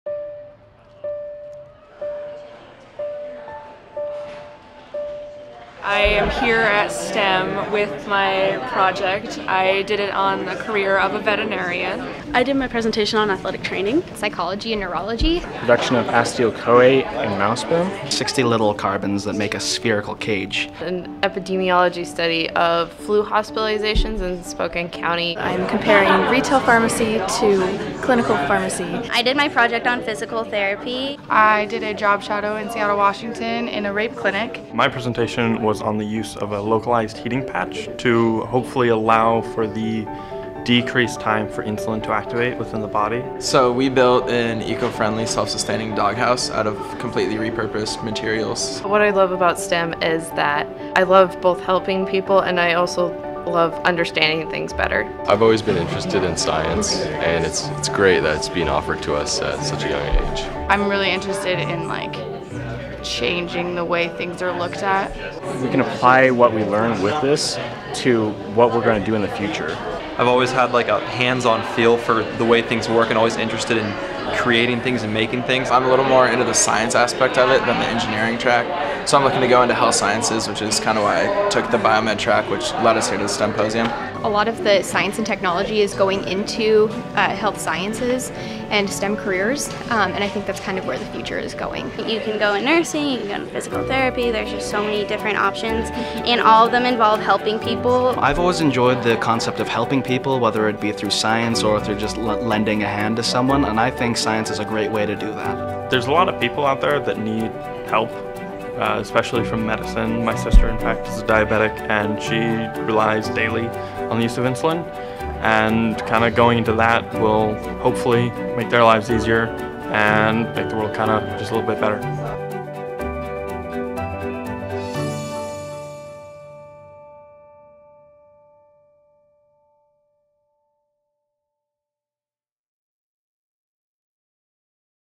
STEMposium presenters: 'Why I'm choosing a STEM path'
Ferris High School students discuss why STEM (Science, Technology, Engineering, and Mathematics) is their chosen career track at the 2015 STEMPosium, which took place May 27, 2015 at the Washington State University Spokane campus.